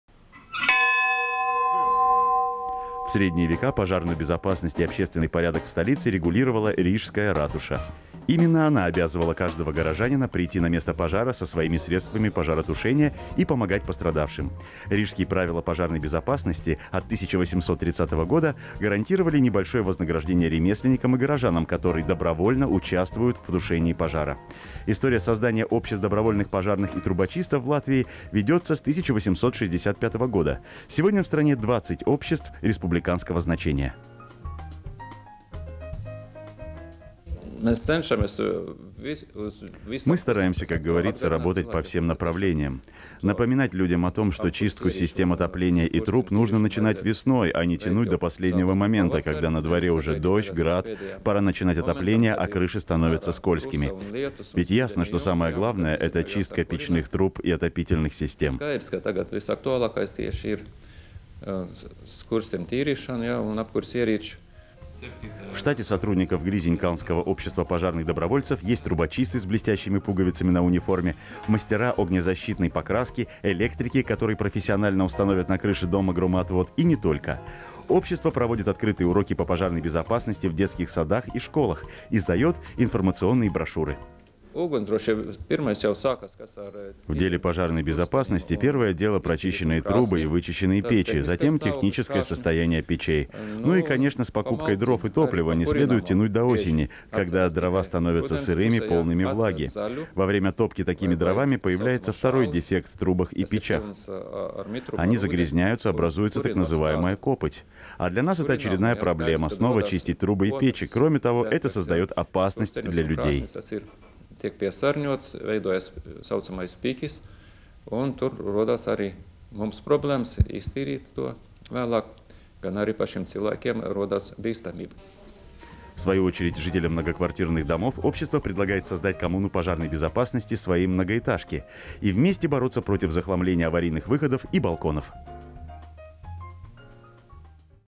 Рижское общество пожарных-добровольцев
В Средние века пожарную безопасность и общественный порядок в столице регулировала Рижская Ратуша. Именно она обязывала каждого горожанина придти на место пожара со своими средствами пожаротушения и помогать пострадавшим. Рижские правила пожарной безопасности от 1830 года гарантировали небольшое вознаграждение ремесленникам и горожанам, которые добровольно участвуют в тушении пожара. История создания обществ добровольных пожарных и трубочистов в Латвии ведется с 1865 года. Сегодня в стране 20 обществ республиканского значения.
Станислов Сунгайла, председатель общества пожарных-добровольцев: Mēs cenšamies ka saka, uz visām pusēm, atgādināt cilvēkiem, ka patiešam to apkures ieriču un skursteņu tirīšanu jāveic sākot no pavasara, nevis sagaidīt līdz pedējam momentam, kad ir arā krusa, un lietus, un slideni jumti un kad visiem jau jāsak kurināt. Bet skaidrs, ka tagad visaktuālāks ir skursteņu tīrīšana un apkures ieriču.

В штате сотрудников Гризинькалнского общества пожарных-добровольцев есть трубочисты с блестящими пуговицами на униформе, мастера огнезащитной покраски, электрики, которые профессионально установят на крыше дома громоотвод, и не только. Общество проводит открытые уроки по пожарной безопасности в детских садах и школах, издает информационные брошюры.
Станислов Сунгайла, председатель общества пожарных-добровольцев: Ugunsdrošībā pirmais jau sākās ar tīriem skursteņiem un iztīrītiem krāsnim, tad tehniskais stāvoklis krāšņu. Nu, un par malciņu arī, par kurināmo -- bieži par to atceras rudens pusē, atved zaļu, vai t.s. šāli, pilnu ar mitrumu, ar ūdeni, un, kurinot tāda veida kurināmo rodas otrais defekts skursteņos un krāsnīs  -- tas tiek piesārņotas, veidojas t.s. piķis un tur rodas arī mums problēma to vēlāk iztīrīt, gan arī pašiem cilvēkiem rodas bīstamība.

 В свою очередь, жителям многоквартирных домов общество предлагает создать коммуну пожарной безопасности своей многоэтажки и вместе бороться против захламления аварийных выходов и балконов.

Рига, ул. Я. Асара, 13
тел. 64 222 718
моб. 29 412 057